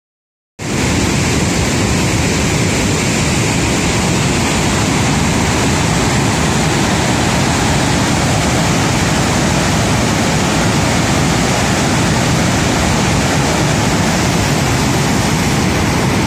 Free B-roll of slurry discharging from an overflow ball mill
Free B-roll panning from a turning ball mill to the overflow discharge lip of the twin ball mill.